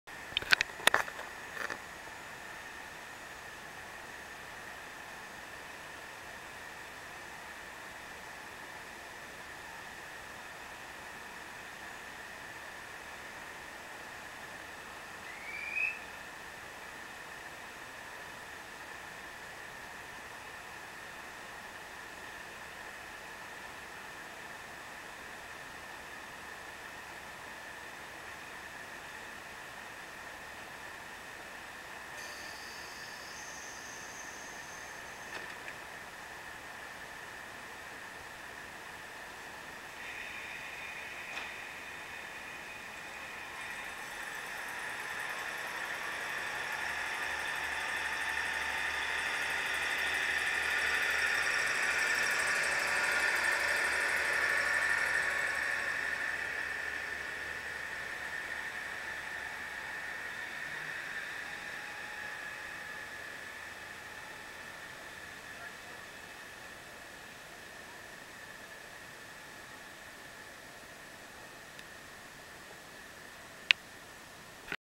石巻線渡波駅 1
HDDを漁っていたら、こんな動画を見つけました。石巻線の渡波駅でDCが発車する所です。この動画は「未来へのキオク」へ投稿しています。